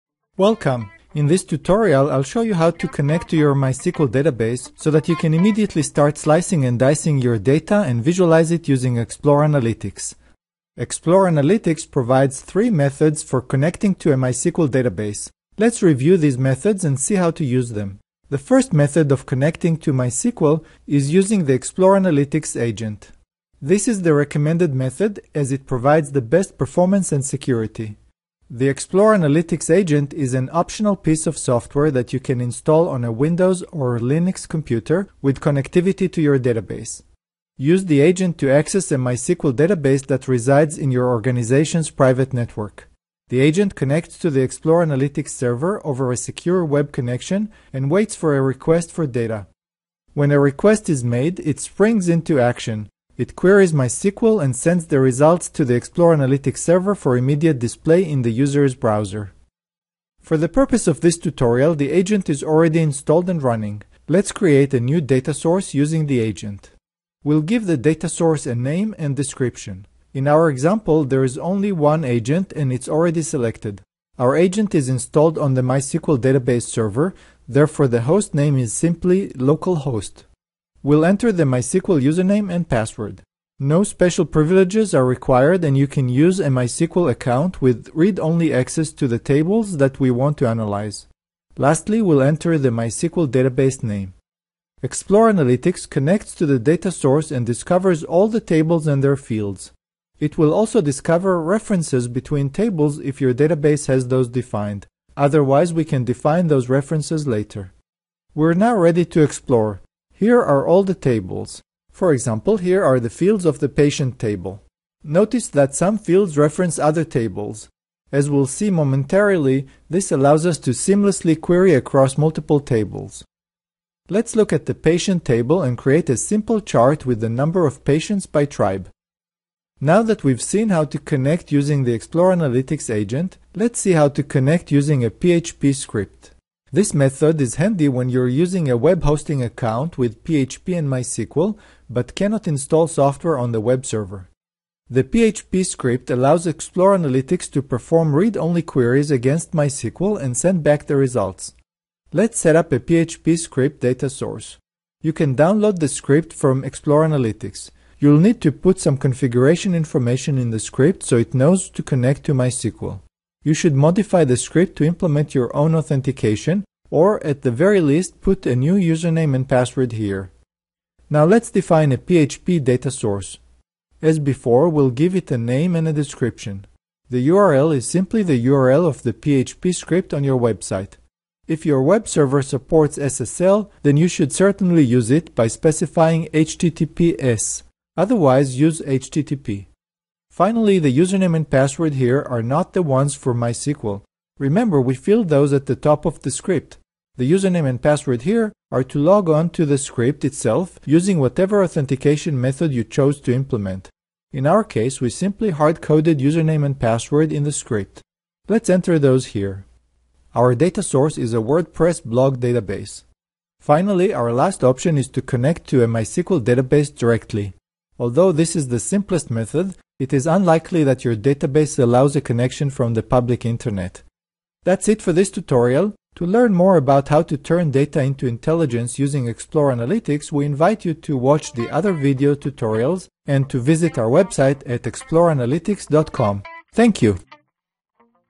MySQL Reporting and Visualization using Explore Analytics
This tutorial explains the different methods of connecting Explore Analytics to MySQL from the cloud. Now you're ready to slice-and-dice and visualize your data!